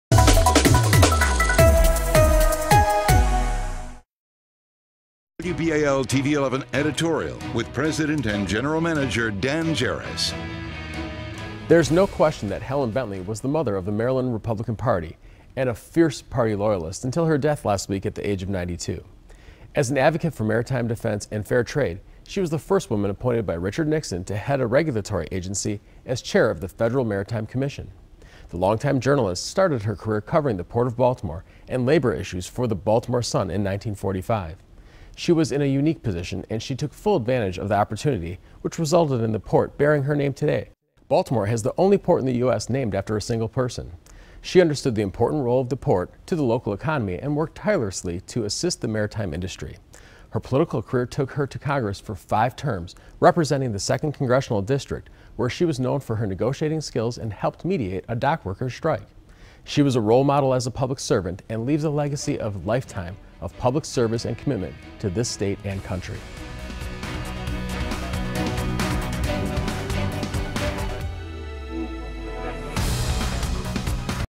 Remembering Helen Bentley
There is no question that Helen Bentley was the mother of the Maryland Republican Party and a fierce party loyalist until her death last week at the age of 92.